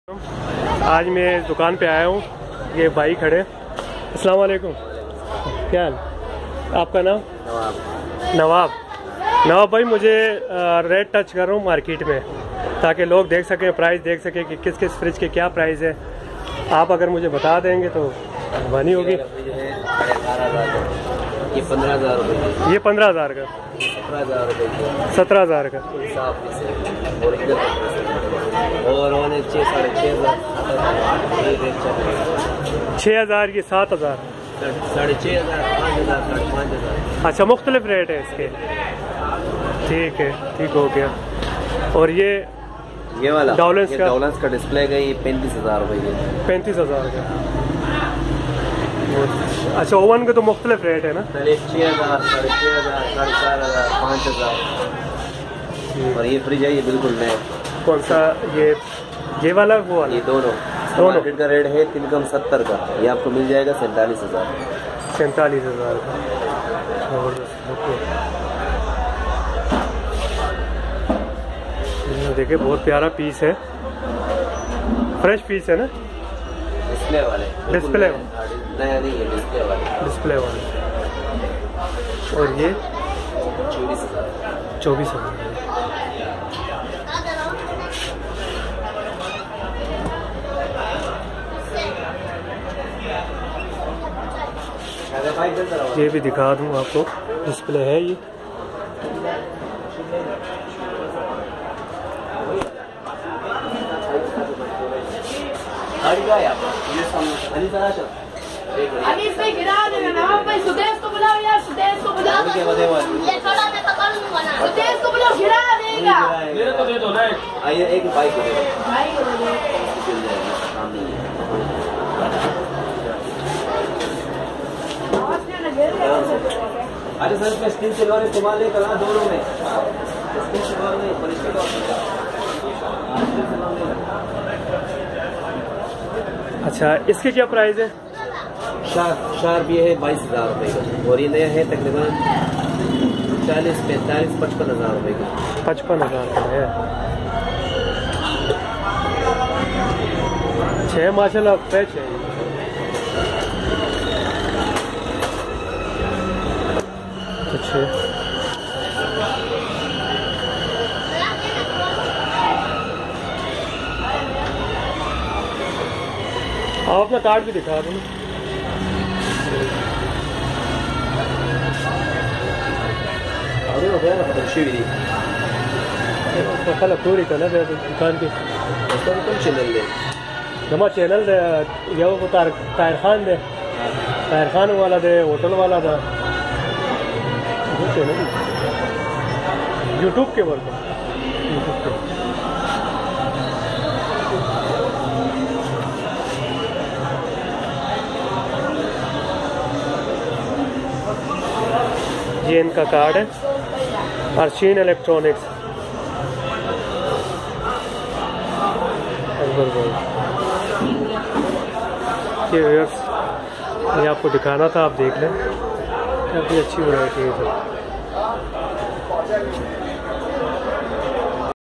Imported Low Price Refrigerator/Oven Stove I 2020 Market Rate Update I Jackson Market Karachi
Imported Low Price Refrigerator/Oven Stove I 2020 Market Rate Update I Jackson Market Karachi 2020,
we visit second hand fridges cheap prices in Jackson market Karachi Sindhi Pakistan, and i hop you like used fridges in very  low price fridges in Jackson electronic market Karachi Sindhi Pakistan.Near nagena center Jackson electronic market Karachi Pakistan.please like and share this video please must be describe my this channel.